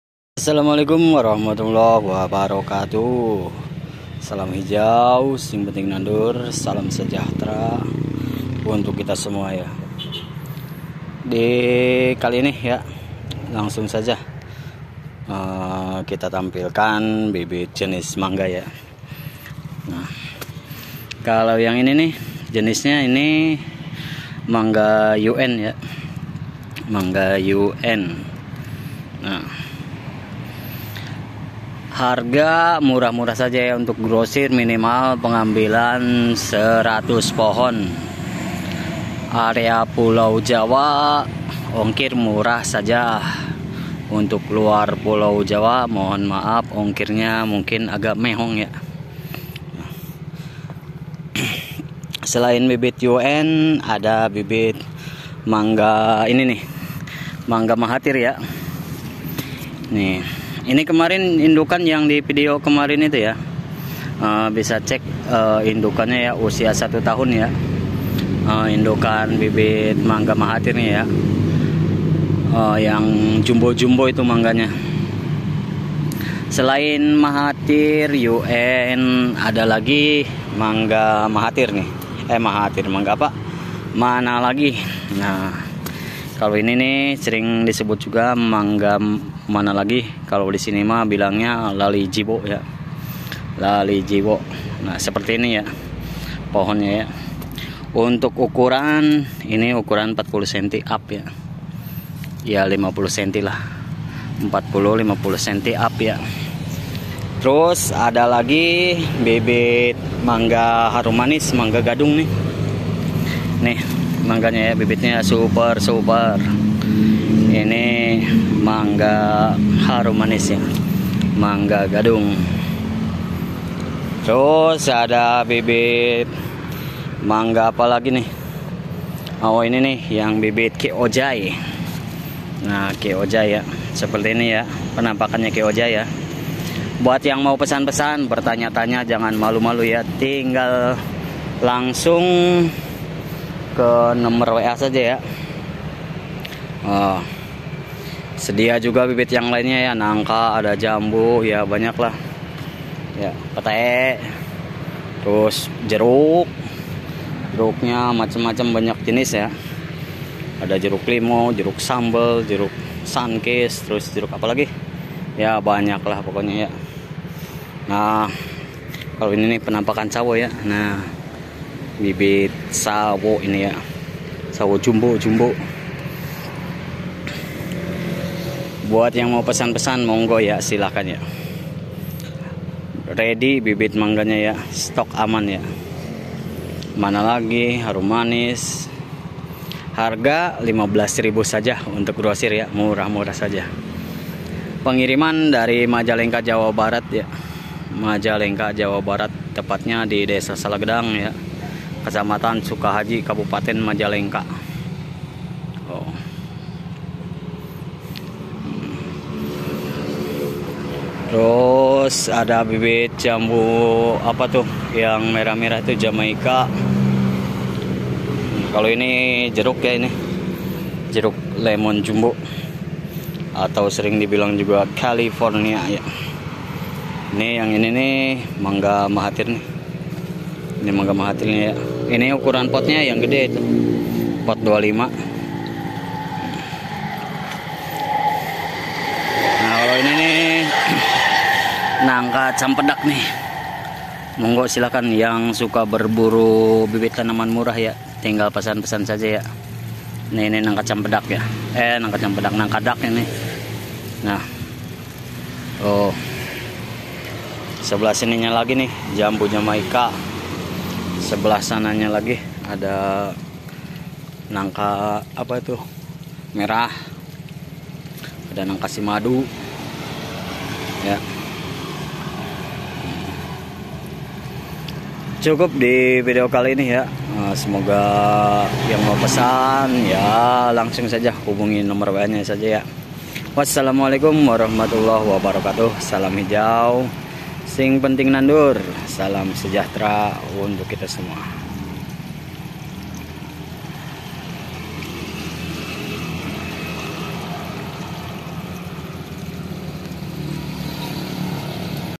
pusat bibit tanaman buah Majalengka || bibit mangga murah || jenis bibit mangga
Bibit Mangga || berbagai macam jenis bibit mangga
cocok untuk di jual lagi

Assalamu'alkaikum ,,salam hijau sing penting nandur.disini saya akan mengenalkan berbagai jenis tanaman buah. jenis tanaman bibit buah.mangga.
Kiojay
Manalagi
Mahatir
Okyong
Harummanis
Cengkir/drmayu
Cokanam
Erwin
Yuen dll
Bibit alpukat
Bibit buah jeruk:;
Nagami
Siam
Siam madu
Kiv
Tongheng
Dekopon
Chokun
Bali
Bali pamelo dll.
Dan masih banyak yg lain ny juga...bila anda berminat silahkan hubungi wa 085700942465..harga ecer dan partai beda ya ,kawan hijau.
ciri-cirijenis
bibit tanaman buah
bibit tanaman buah
Bibit nangka;
Simadu,sidulang,oren.mini..nangkadak,campedak.